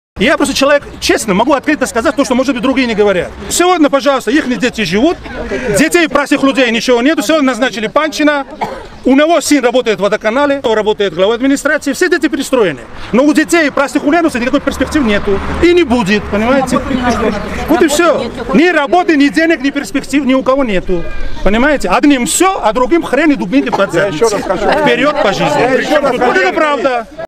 👉 Матвей Володарский: одним всё а другим хрен и дубинкой под з******
👉 Их арестовала ФСБ!
- Советника губернатора Ульяновской области Эделя и мэра Димитровграда Большакова 8 августа арестовали и отправили в тюрьму.
✔ Матвей Володарский:
"Убежден, что за взятки своих подчинённых - надо арестовывать самих губернаторов"!